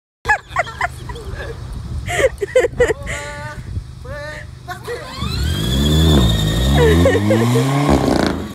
Kid on Toy Motorbike Challenges Driver to a Race || ViralHog
Occurred on 01/01/2022 / France

Funny lesson, all public.